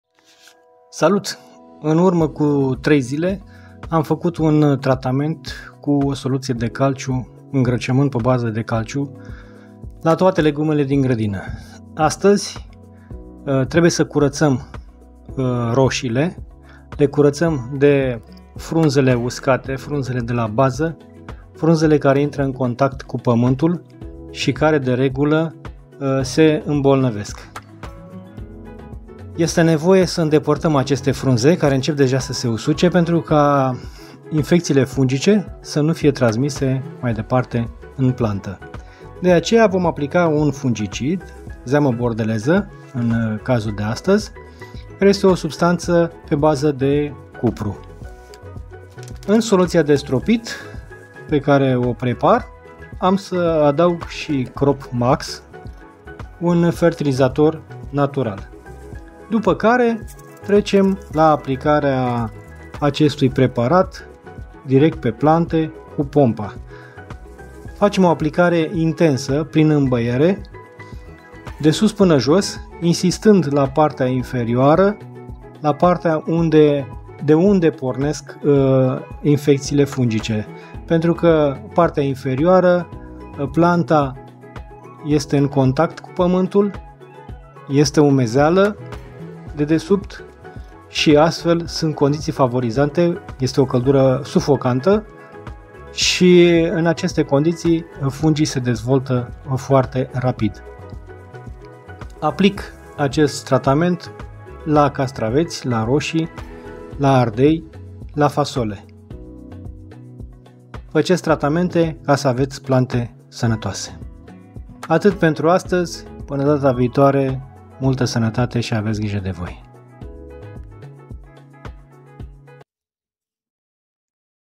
Curățarea roșiilor de frunzele mănate- tratament .
Frunzele de la baza rosiilor care intra in contact cu pamantul sunt prima linie de atac a fungilor. Indepartarea acestor frunze afectate limiteaza extinderea bolii in planta.Este necesara aplicarea unui tratament antifungic cu o solutie pe baza de cupru - zeama bordeleza dar si un fertilizator foliar - cropmax.